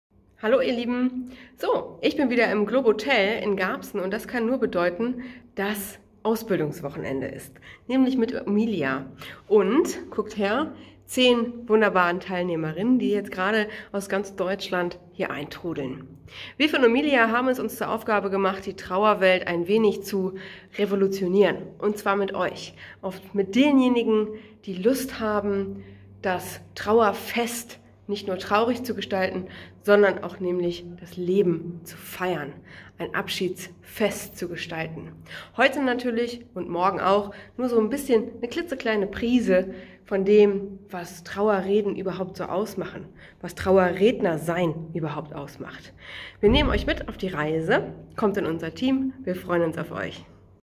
✝️Ein kleiner Einblick in unser Ausbildungswochenende Abschied ✝️
Nadine Weske, Ausbilderin bei Omilia freut sich darauf, Ihr Wissen mit angehenden Trauerredner zu teilen.